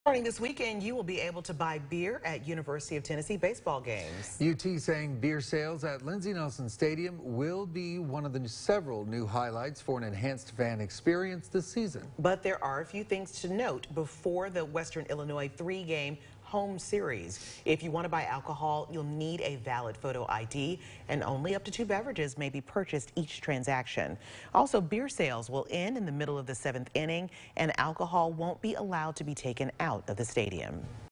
Vols Baseball + Beer = New fan experience at Lindsey Nelson Stadium
The University of Tennessee announced that beer sales at Lindsey Nelson Stadium will be one of several new highlights for an enhanced fan experience for the 2020 Tennessee baseball season.